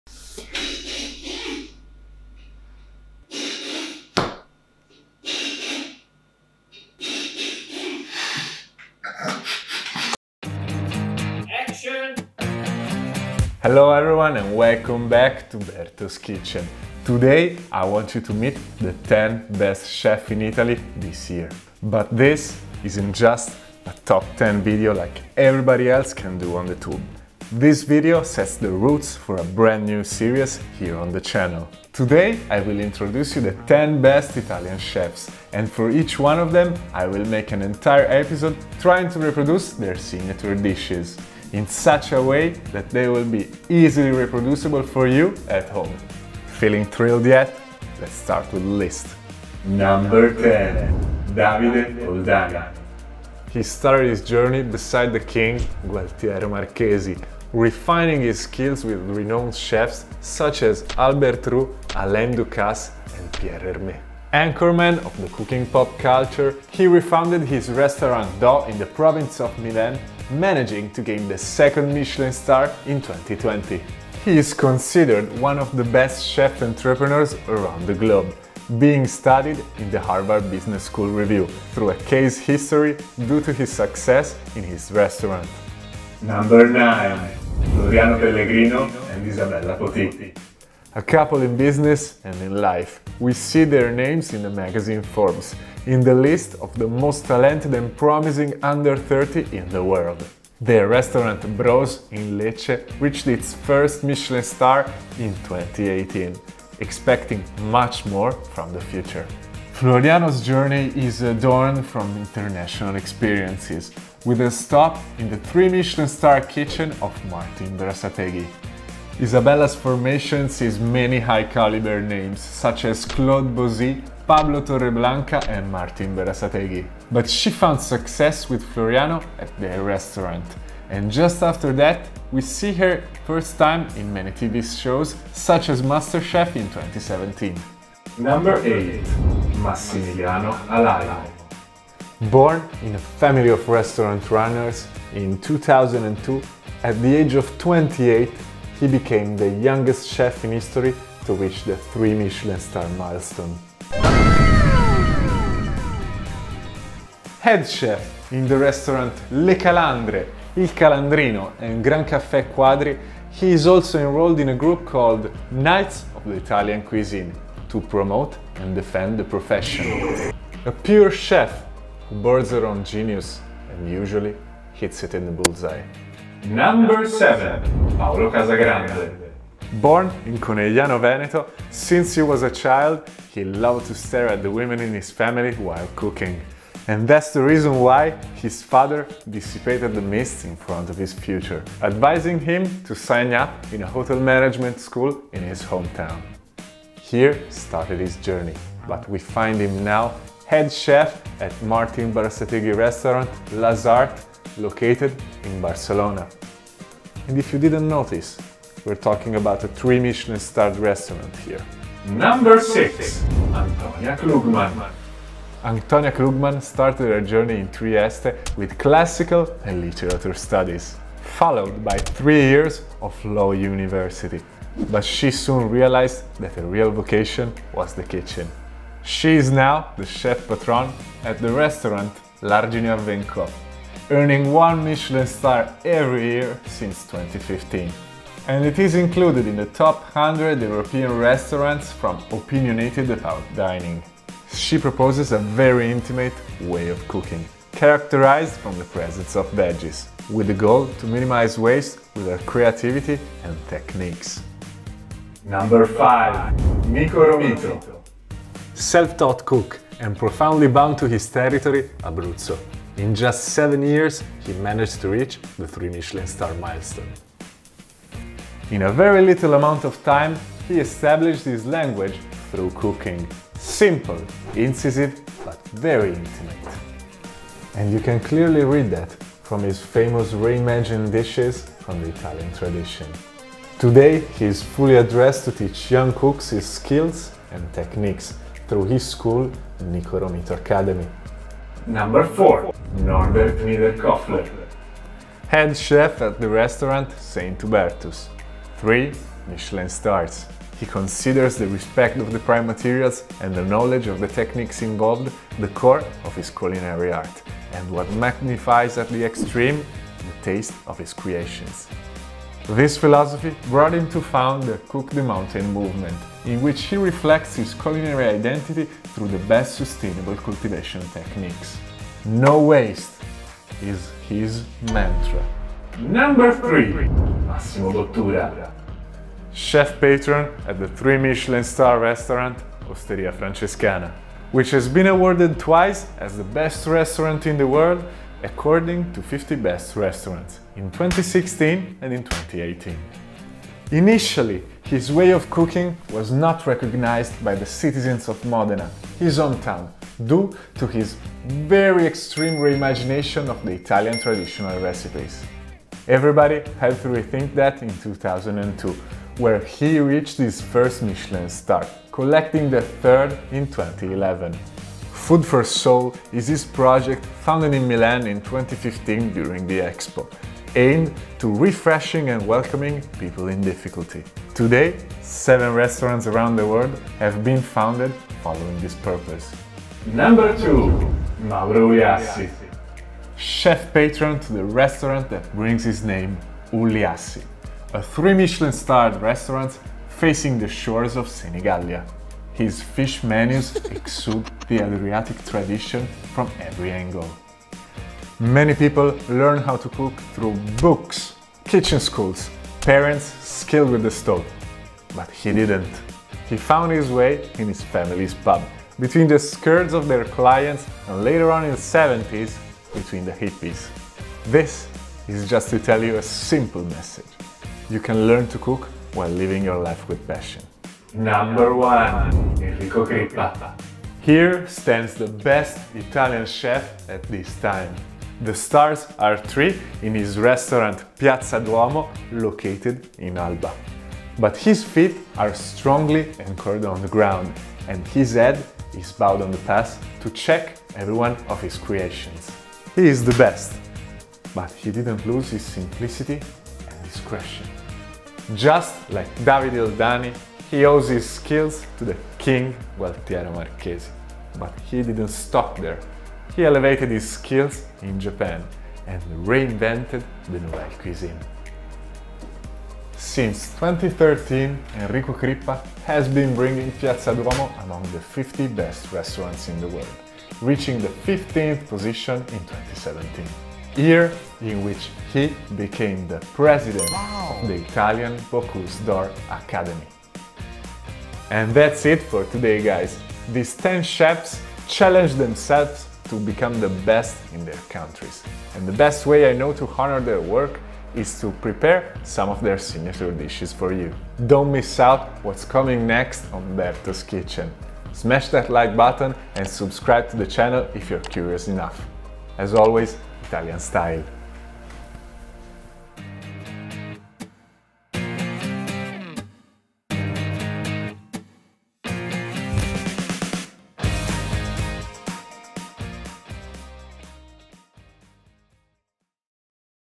TOP 10 Best chefs in Italy 2021 | NEW SERIES
Meet the 10 best chefs in Italy!
This video sets the roots for a brand new series, I will reproduce a signature dish for each one of these top chefs!
Do you agree with this list of the TOP 10 Italian Chefs? Which one do you think is the TOP CHEF?

00:48 Davide Oldani
01:25 Floriano Pellegrino & Isabella Potì
02:17 Massimiliano Alajmo
02:55 Paolo Casagrande
03:30 Antonia Klugmann
04:14 Niko Romito
04:51 Norbert Niederkofler
05:25 Massimo Bottura
06:19 Mauro Uliassi
07:00 Enrico Crippa